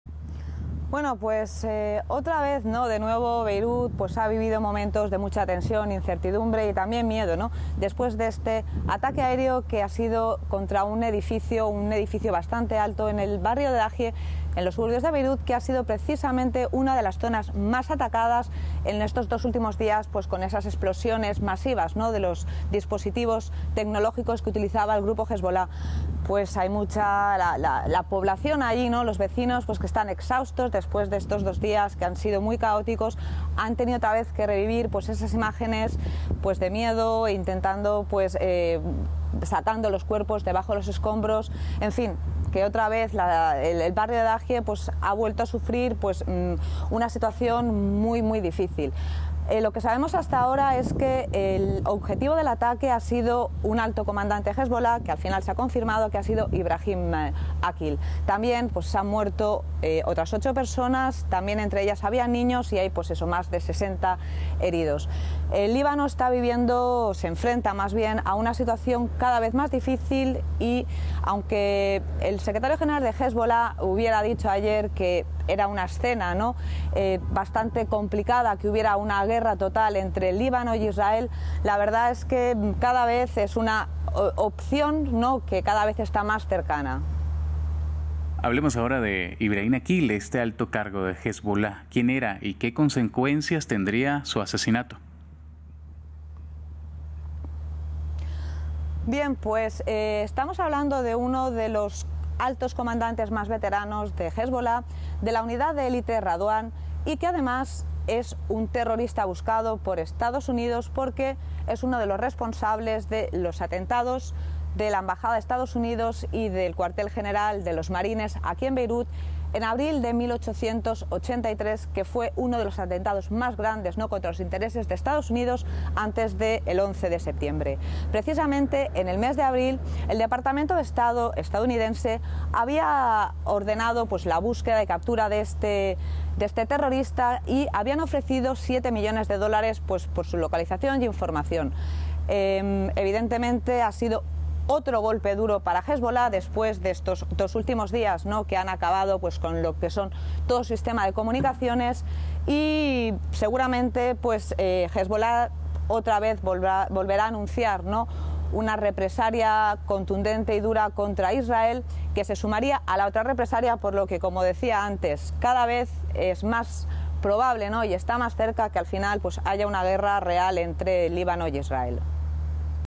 Informe desde Beirut: Israel confirma ataques a la capital libanesa
El Ejército de Israel confirmó este viernes que llevó a cabo un "ataque selectivo" en la capital libanesa, Beirut, solo un día después de que el líder de Hezbolá, Hassan Nasrallah, afirmara que Israel cruzó "todas las líneas rojas", tras acusar al país de las letales explosiones de dispositivos electrónicos en Líbano esta semana. Nuestra corresponsal Ethel Bonet nos dio los detalles de los ataques.